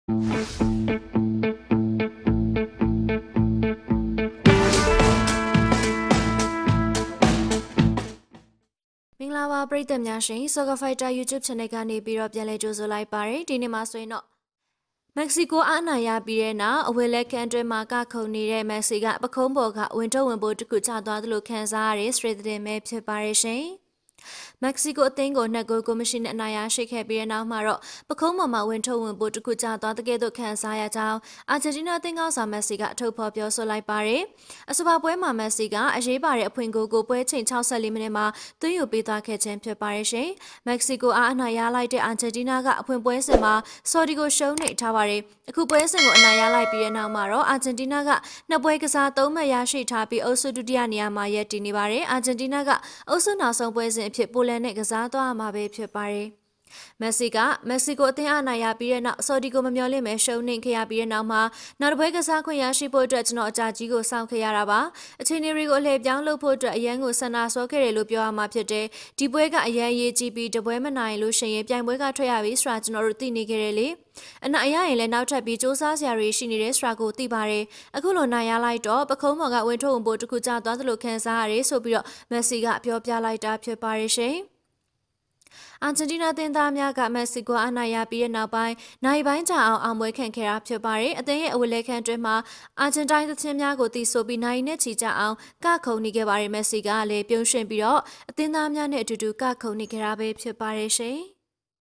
Mexico အသင်းကိုနိုင်ပြီး အဝတ်လဲခန်းထဲမှာ ကခုန် နေတဲ့ မက်ဆီ
Soccer Fighter Official မှ ဘောလုံးသတင်းများ ပုံမှန် တင်ဆက်ပေးနေပါတယ်။